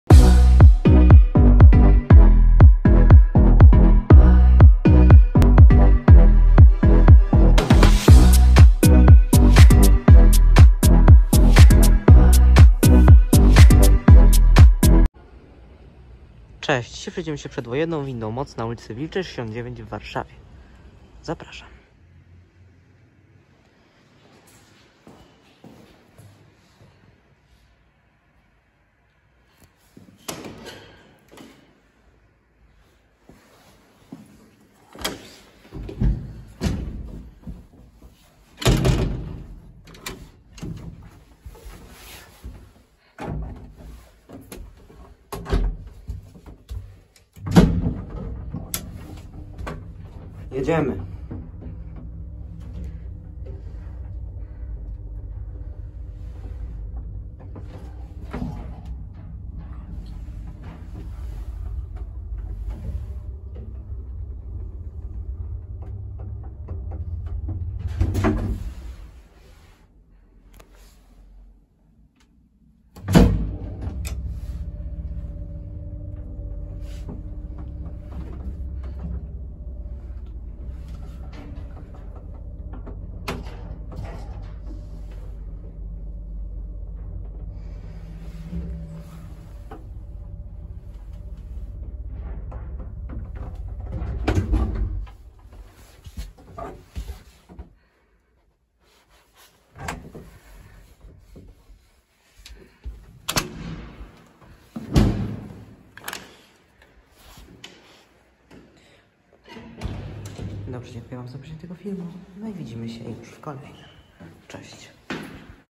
Przedwojenna winda MOC | Wilcza 69 Warszawa
╔═ Informacje ═╗
  • Lokalizacja: Wilcza 69 , Warszawa, Polska
  • Producent: MOC • Rok: 1938
  • Modernizacja: ZUD • Rok 1965
  • Konserwacja: M.P.R.D.O
  • Udźwig: 4 osób lub 320 kg
  • Kondygnacje: 5 (0,1,2,3,4)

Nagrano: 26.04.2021